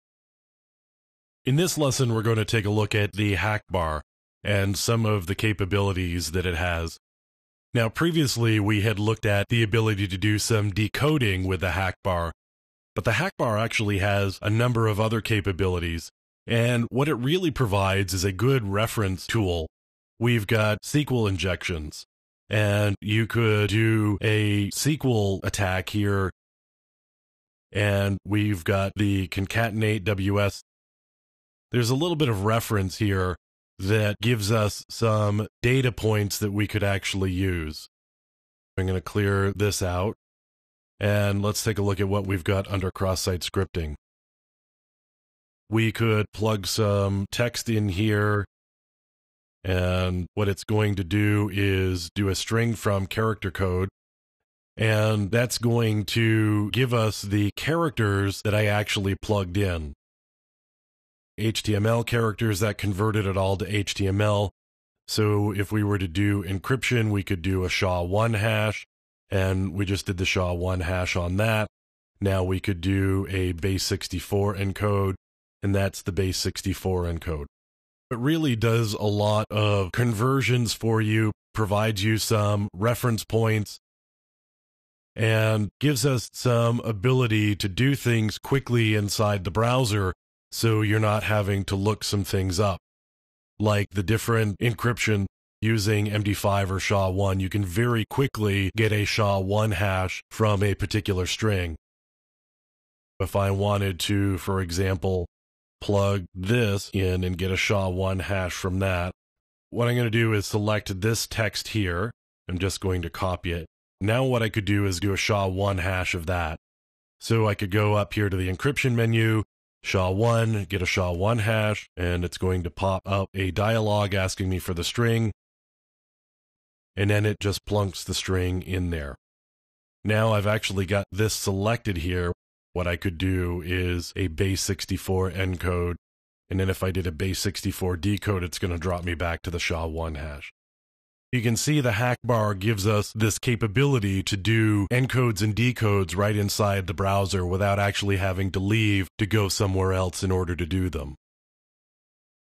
Hackbar Best Plugin For Wbe Pentration
What is hack bar ?
How to use Hack Bar?
Benefits of hack Bar